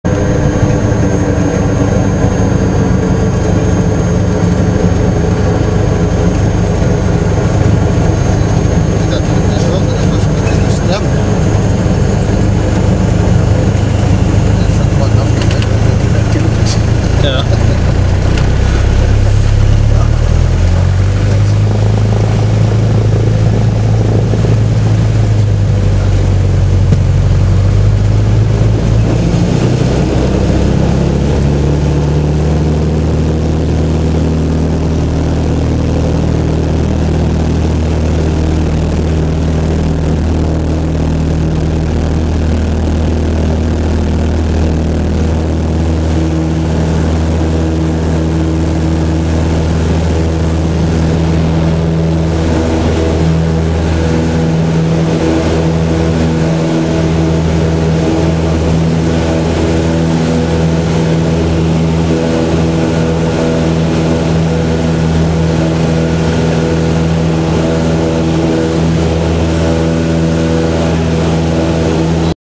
242. ENSG, Cockpit view, Take off 24 LN-DAH, Sogndal, 30.08.2010
242. ENSG, Cockpit view, Take off 24 Sogndal, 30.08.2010
Sogndal lufthavn, Haukåsen, Elevation AMSL 498 m / 1634 ft
06/24 1,111 m, 3,645 ft. Asphalt